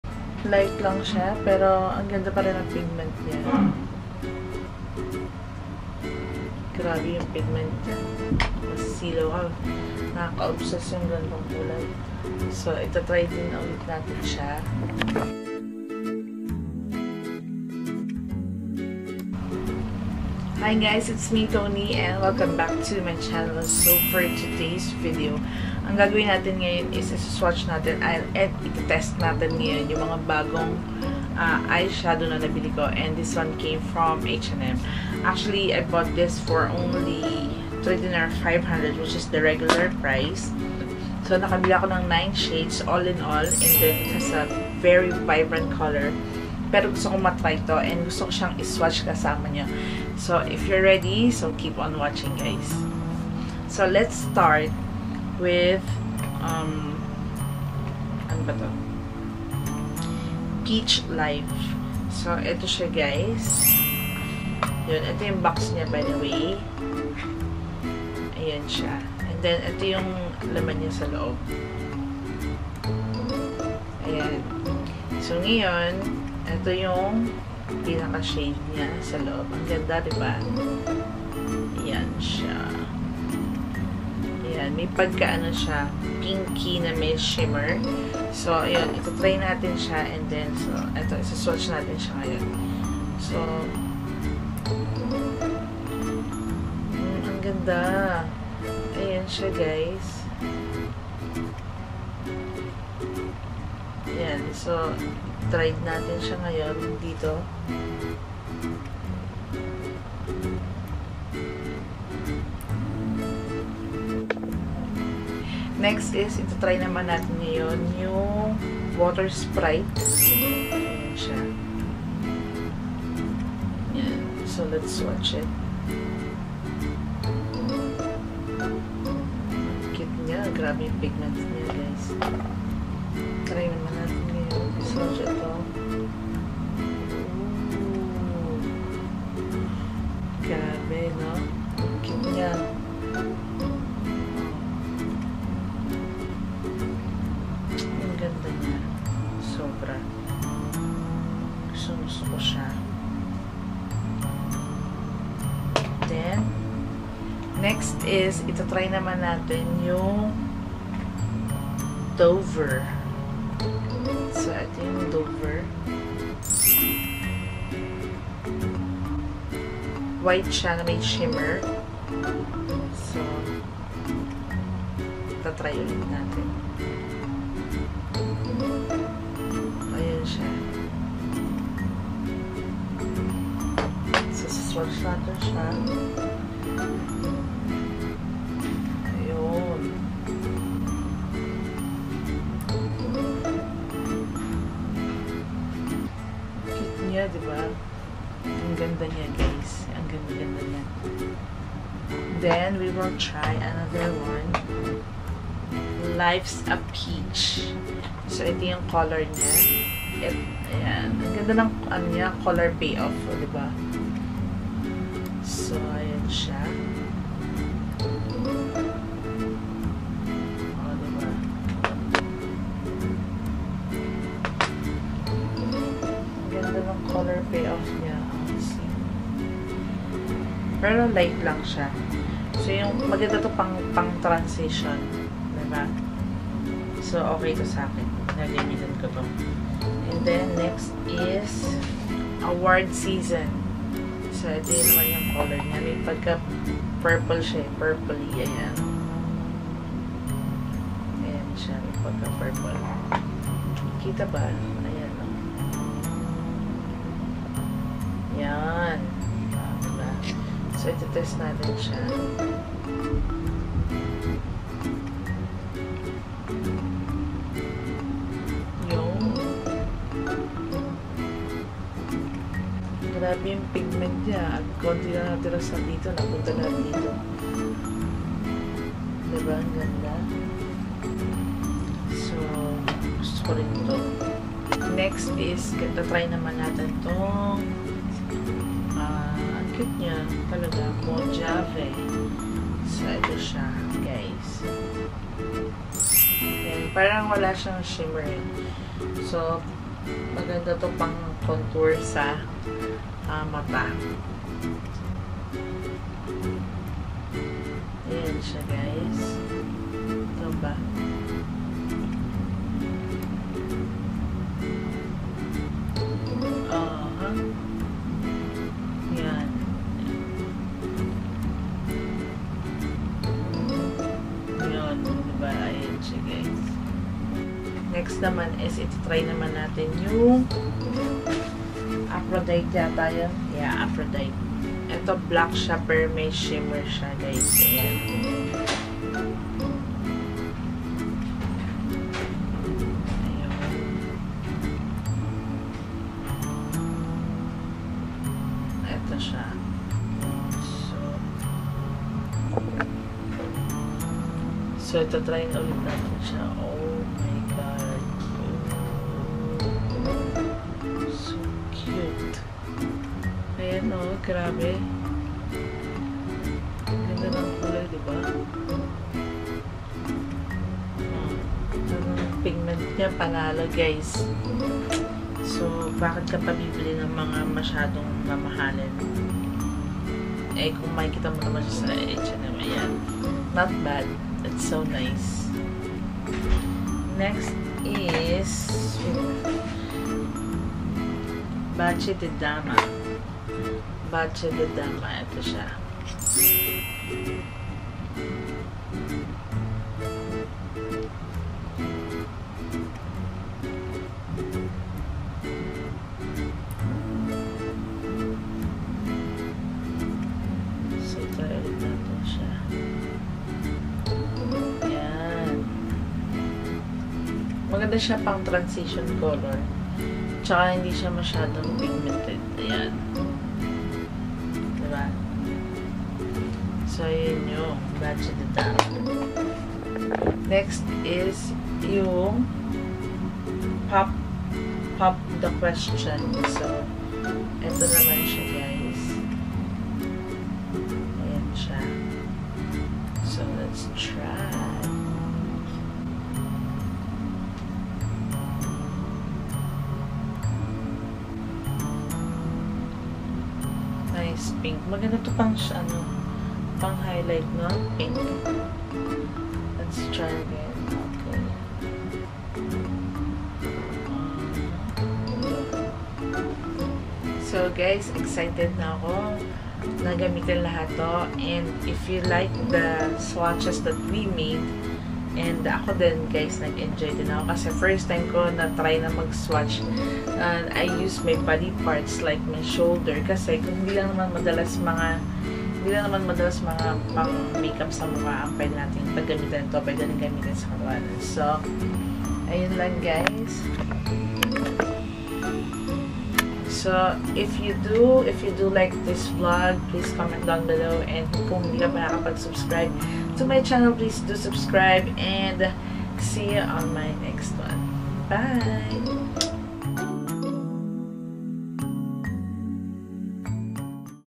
H&M EYESHADOW POT SWATCHES AND REVIEW | LADY ANTONETTE
I am so obsessed about this and so excited to show you all the different vibrant colors from H&M.